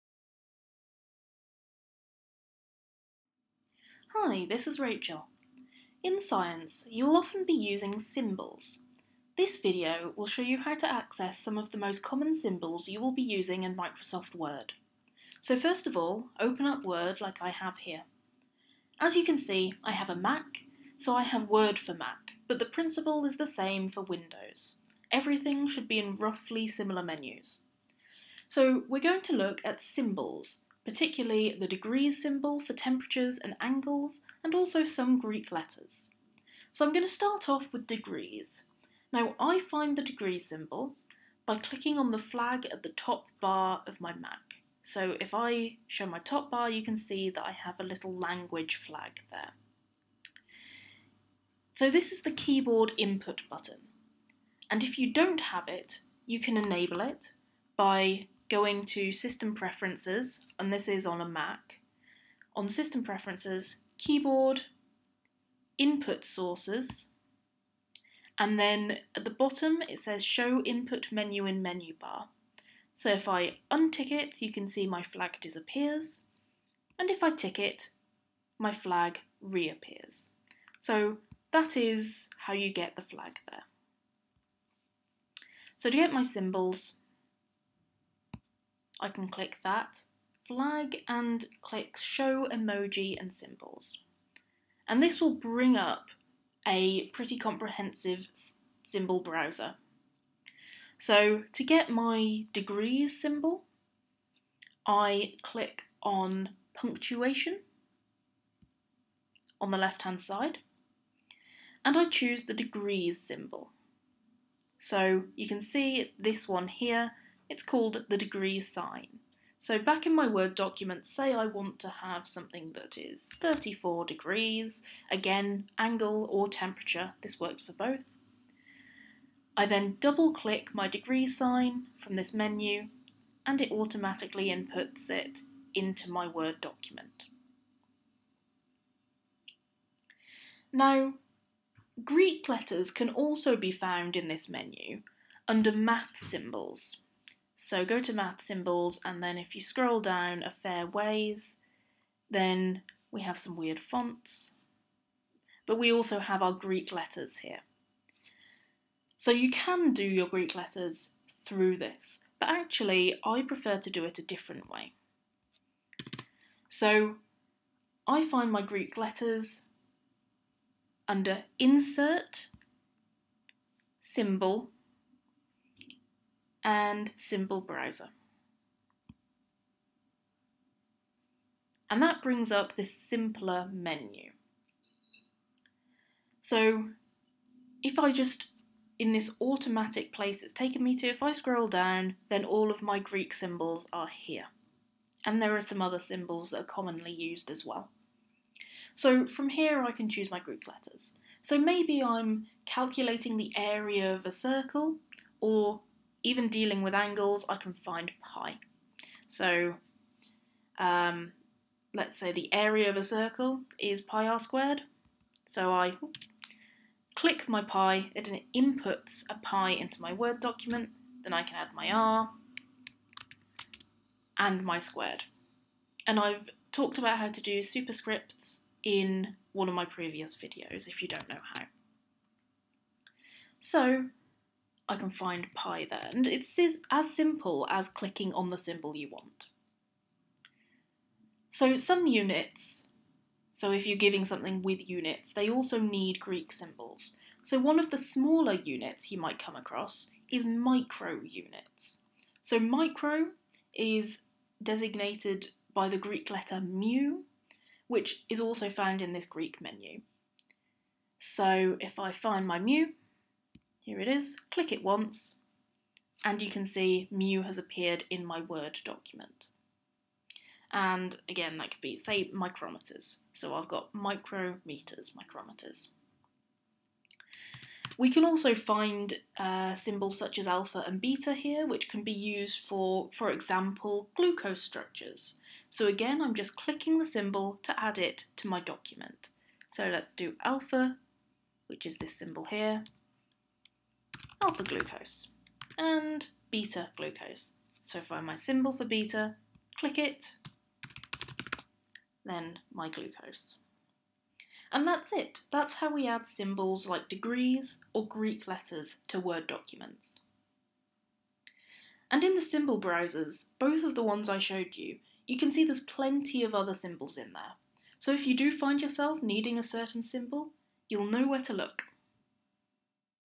Symbols for science in Microsoft Word
Rachel explains how to find symbols in Microsoft Word. This is aimed primarily for those in the sciences, but is also applicable to other subjects that use symbols.